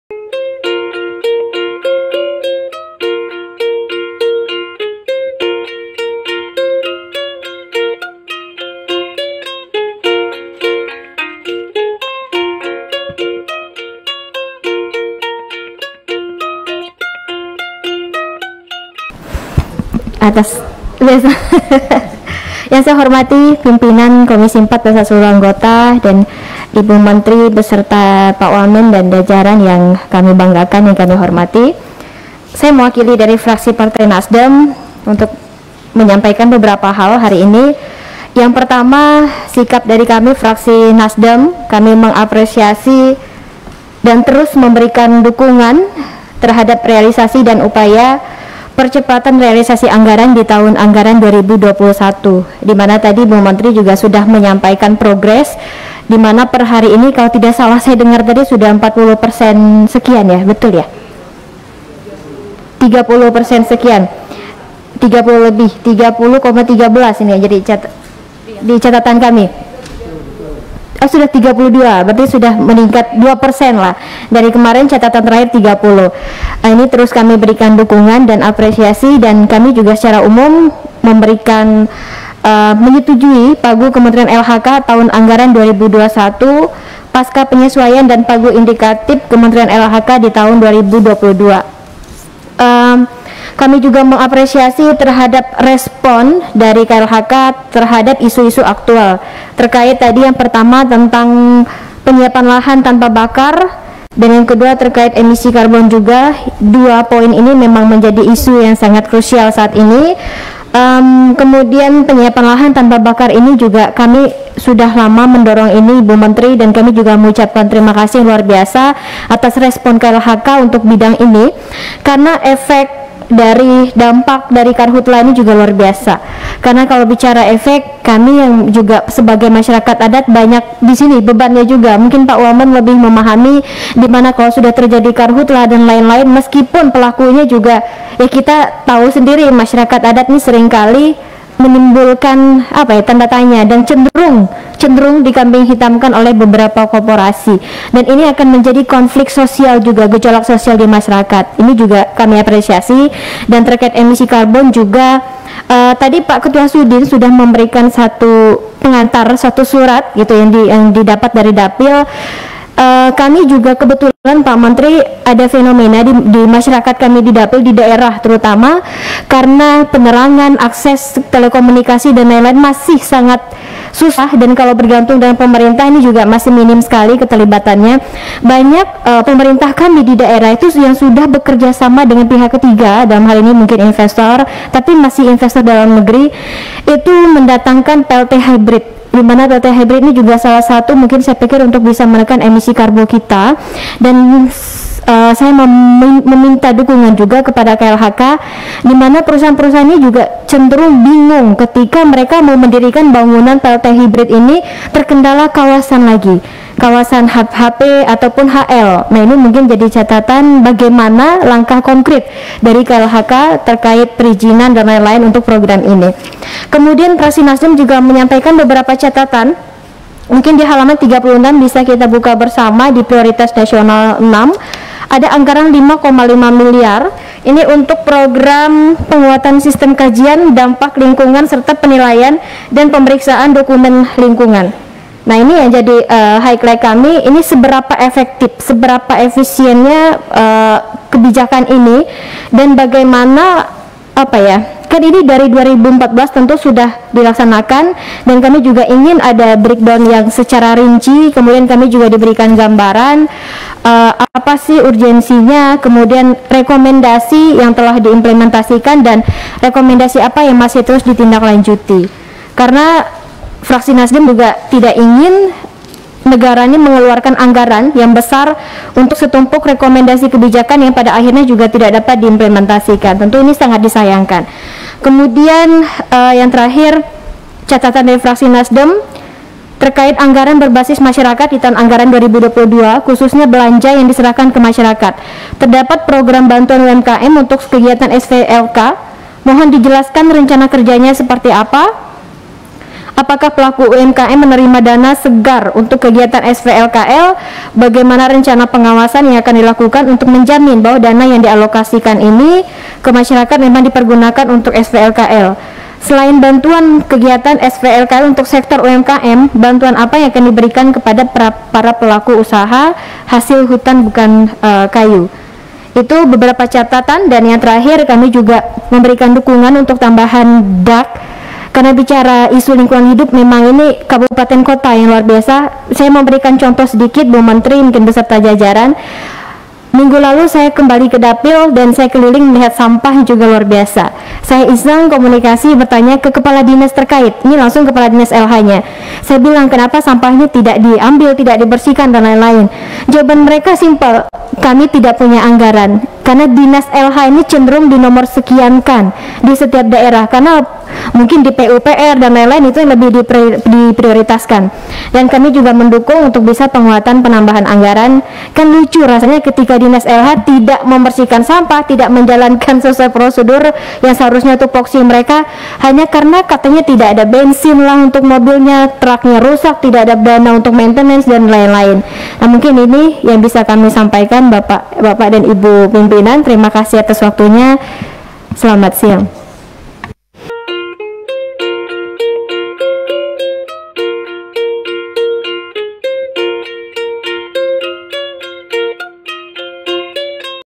Suara Politisi Cantik Nasdem di DPR RI - Siapkan Lahan Tanpa Bakar
Politisi Canti NasDem menyampaikan beberapa pandangannya saat Komisi IV DPR RI Rapat Kerja dengan Menteri Lingkungan Hidup dan Kehutanan RI, terkait tindak lanjut Rapat Kerja tanggal 10 Juni 2021, Rabu, 23 Juni 2021

termasuk penyiapan lahan tanpa melalui proses pembakaran